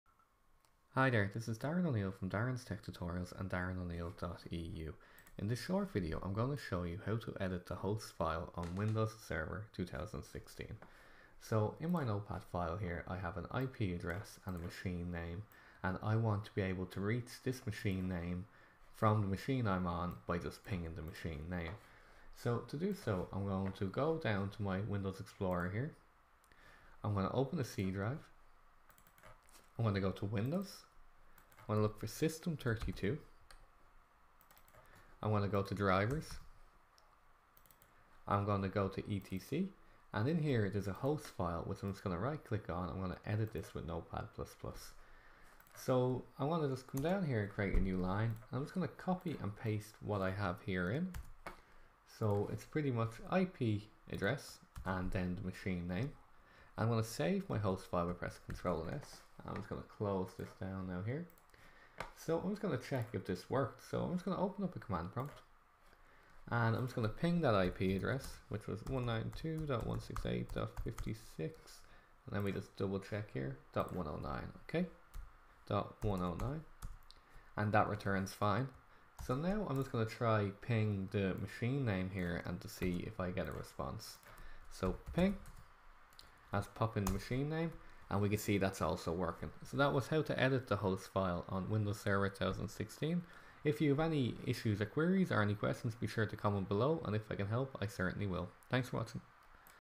How to edit the hosts file on Windows Server 2016
This hosts file tutorial will show you how to edit the hosts file on windows server 2016 although the process needed to edit the hosts file on windows 10 should be much the same.

In this video you will first see the location of the hosts file on windows.

C:\Windows\System32\drivers\etc\

You will then edit the hosts file adding an IP address and hostname to it .

You will the save and exit the hosts file.

Finally you will test that your changes have been successful by pinging the name you set in your hosts file to insure you get a response.